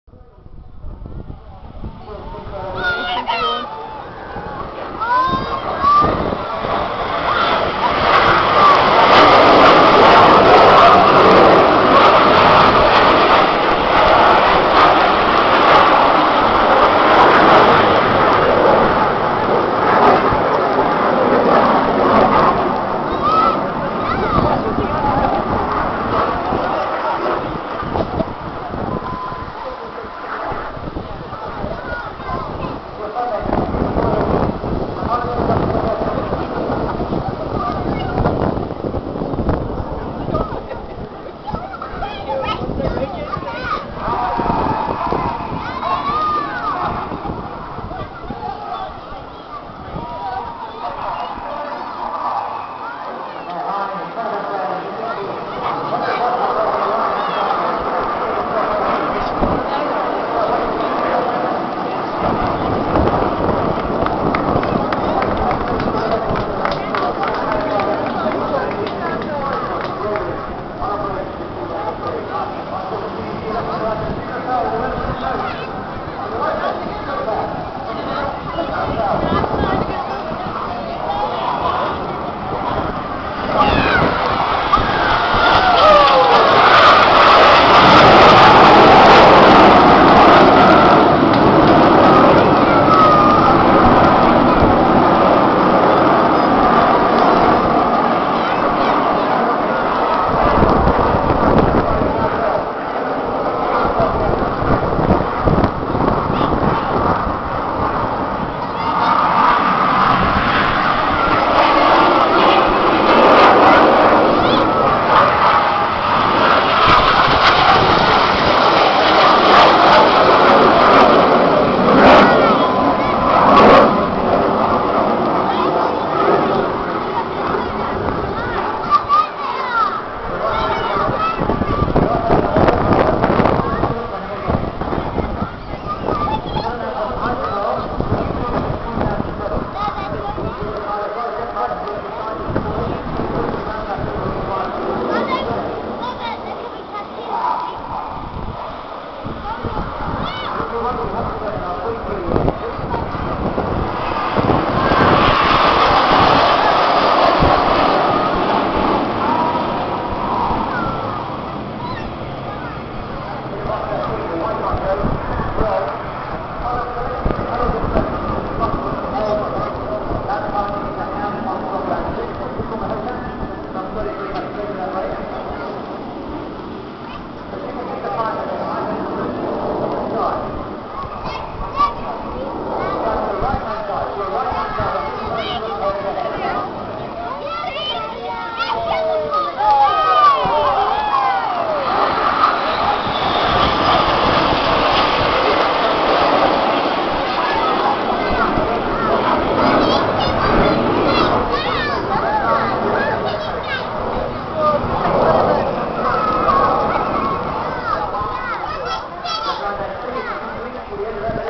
Red Arrows Display
The Red Arrows Display in Lifeboat Week 2007, Lyme Regis. The sun came through just in time.